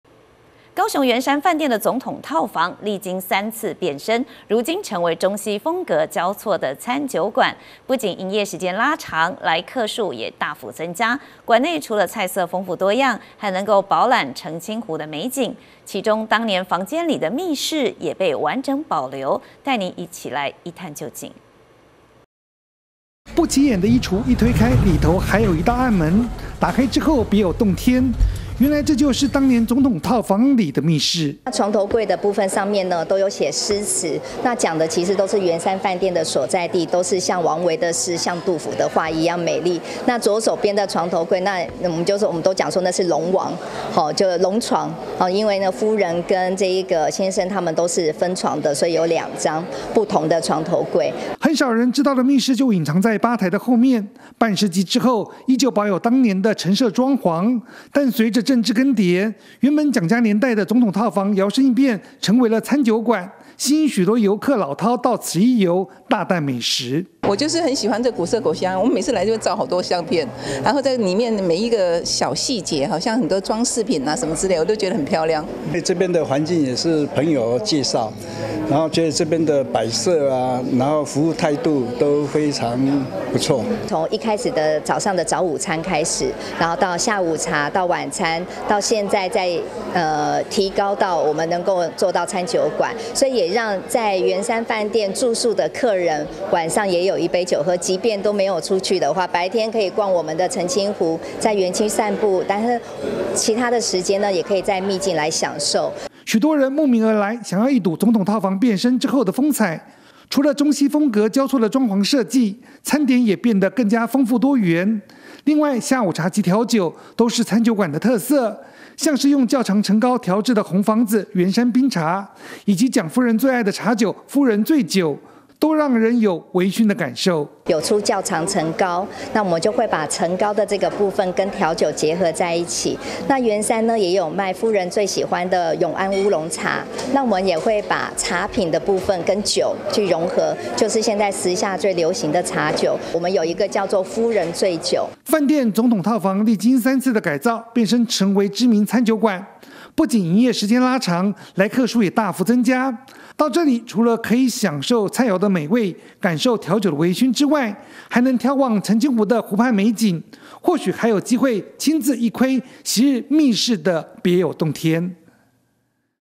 總統套房改造變身 秘境餐酒館藏密室│T-NEWS聯播網
高雄圓山飯店的總統套房，歷經三次變身，如今成為中西風格交錯的餐酒館，不僅營業時間拉長，來客數也大幅增加，館內除了菜色豐富多樣，還能飽覽澄清湖的美景，其中，當年房間裡的密室也被完整保留，帶您來一窺究竟。